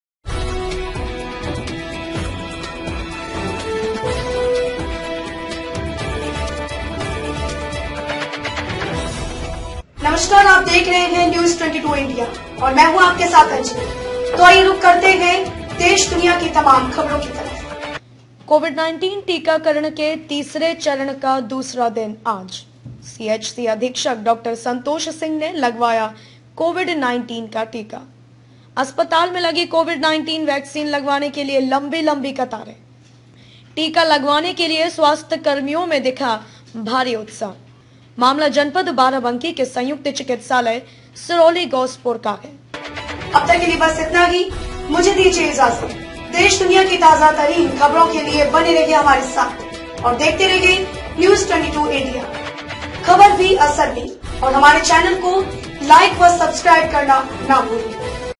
सीएचसी अधीक्षक डॉक्टर संतोष सिंह ने लगवाया कोविड़ 19 का टीका।
कोविड-19 टीकाकरण के तीसरे चरण का दूसरा दिन आज।

अस्पताल में लगी कोविड़ 19 वैक्सीन लगवाने के लिए लंबी लंबी कतारें।

टीका लगवाने के लिए स्वास्थ्य कर्मियों में दिख भारी उत्साह।

मामला जनपद बाराबंकी के संयुक्त चिकित्सालय सिरौलीगौसपुर का।